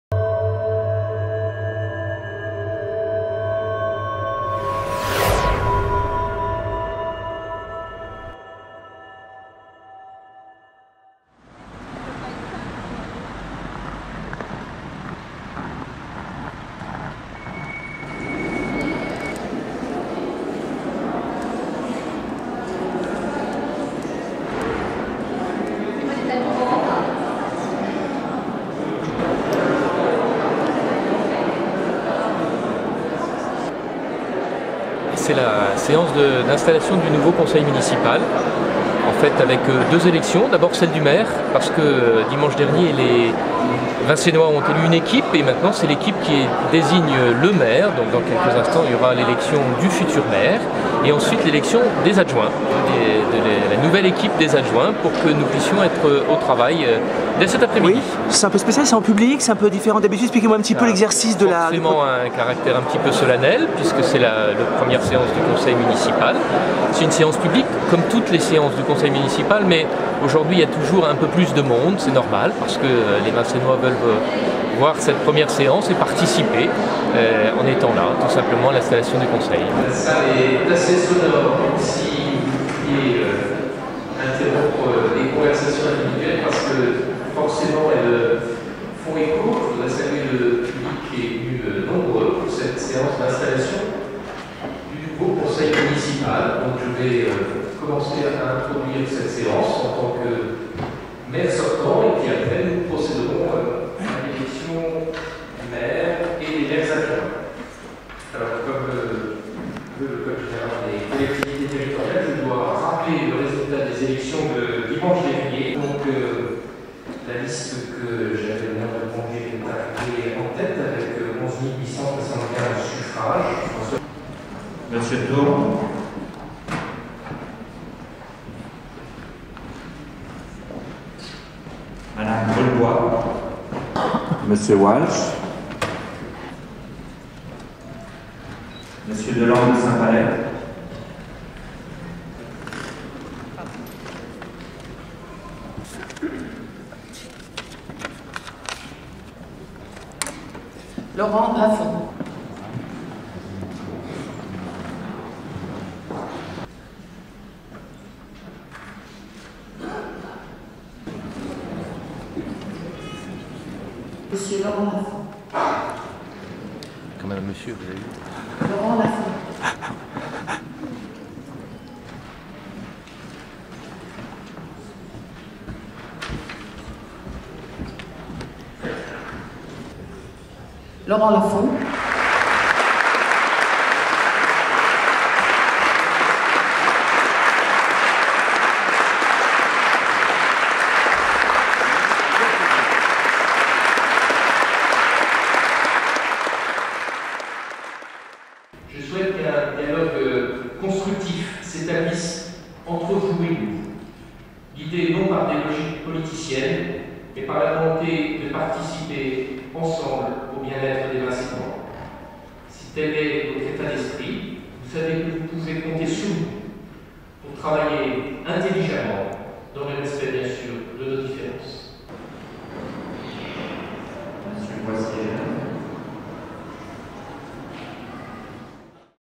Dans les coulisses des élections municipales à Vincennes Laurent Lafon réélu
Dans les coulisses des élections municipales à Vincennes.